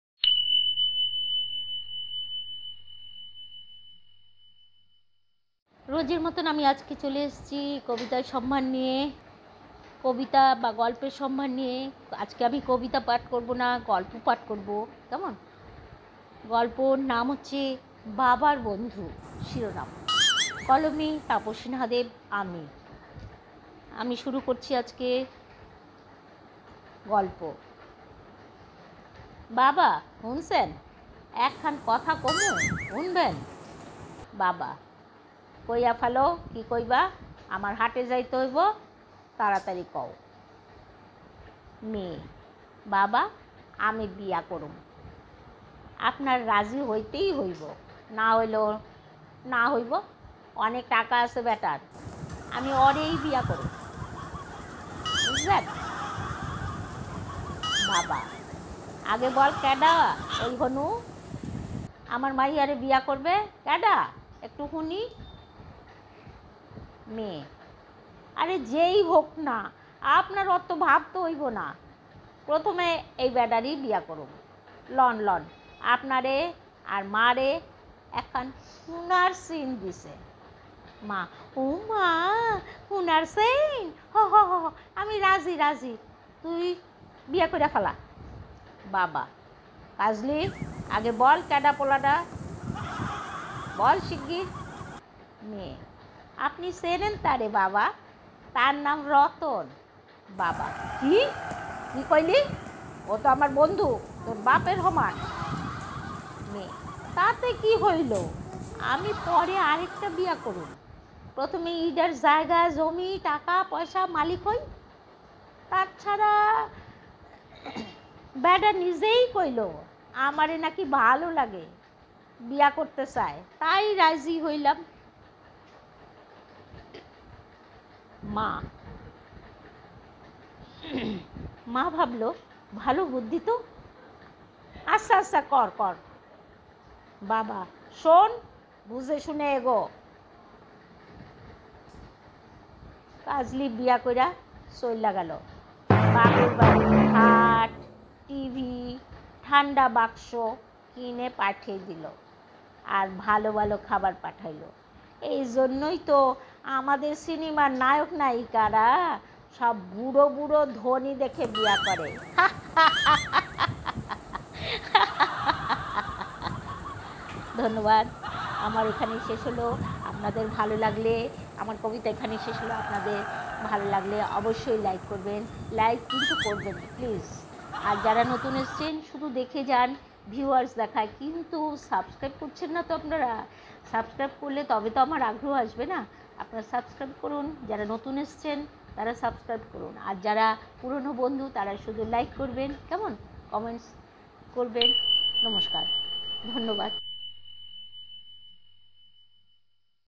Babar bandhu ,❤️ writers-Tapasi naha Deb. also voice. bhishon hashir.😁😁😃😃
--    বাবার বন্ধু ---
 কলমে --- তাপসী নাহা দেব ।

 বাবা হুনছেন  এক খান কথা কমু , হুনবেন ?

 বাবা- কইয়া ফেল, কি কবি , আমার হাটে যাইতে হইবো ,  তাড়াতাড়ি ক ।
 মেয়ে-  বাবা আমি বিয়া করুম ,  আপনার  রাজি
হইতে হবে,  না হইলে না হবে , অনেক টাকা আছে বেটার , আমি ও রে ই বিয়া করুম ।
বাবা-   আগে বল কেডা  ওই‌ হনু ?  আমার মাইয়ারে বিয়া করবে?  কেডা  - - একটু হুনি।
 মেয়ে--- আরে  যে ই হোক , আপনার   অত্তো     ভাবতে  হইবো  না ,   প্রত্থমে - এই  বেডারে   বিয়া করুম ,
 লন লন আপনারে আর.   মা রে  এক খান সুনার   চ্যাইন দিছে ।
মা ওমা !! হুনার চ্যাইন ? হ হ আমি রাজি আমি রাজি , তুই বিয়া ক ইরা ফালা ।

     বাবা - কাজলী আগে বল কে ডা ,  পোলাডা ?

মেয়ে-   আপনি চেনেন তারে , তার নাম রতন ,
  বাবা-  কি ই ই ? কি ক ইলি?  ও তো আমার বন্ধু ,
 তোর বাপের সমান ,

মেয়ে--  তাতে কি হ ইলো  ?  আমি পরে আর একটা বিয়া করুম , পত্থমে ইডার জমিযামার  টাকা পয়সার মালিক হ ই ।
 তাছাড়া বেডা নিজেই ক ইলো আমারে নাকি ভালোলাগে , বিয়া করতে চায় , তাই রাজি হইলাম।

   মা  - ভাবলো ভালো বুদ্ধিতো !! আচ্ছা আচ্ছা কর  ,
  বাবা -   শোন  বুঝে শুনে এগো ।
  কাজলী বিয়া ক ইরা চ ইল্যা গেলো ।
বাপের বাড়ি  খাট টভি  ঠান্ডা বাক্স  কিনে পাঠিয়ে দিলো । আর ভালো ভালো‌ খাবার  পাঠালো ।
  এইৎজন্য ই তে আমাদের সিনেমার নায়ক নায়িকারা  সব বুড়ো বুড়ো   ধনী  দেখে বিয়ে করে।
  হা হা হা হা হা ।